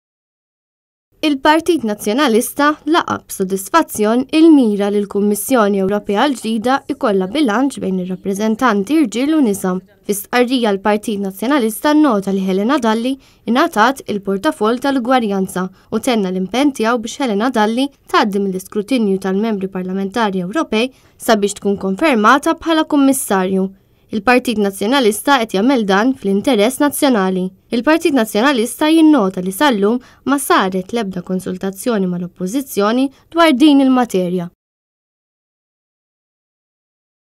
Jonqos il-vot tal-Membri Parlamentari Ewropej
Fi stqarrija, il-Partit Nazzjonalista nnota li Helena Dalli ngħatat il-portafoll tal-ugwaljanza, u tenna l-impenn tiegħu biex Helena Dalli tgħaddi mill-iskrutinju tal-Membri Parlamentari Ewropej sabiex tkun konfermata bħala Kummissarju.